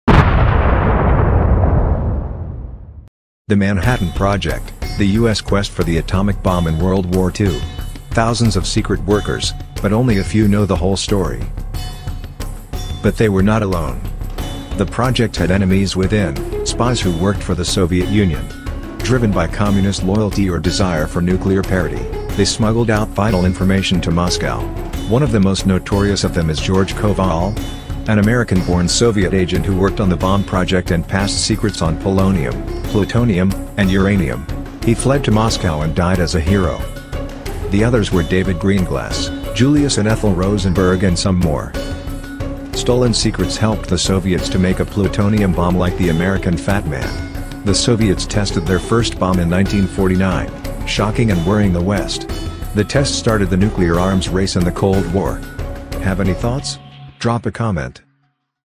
The Atomic Spies: How a Soviet Spy Ring Cracked the Manhattan Project 🔥😎💀
credit to
DocumentsTalk.com
TRK Zvezda
ZNF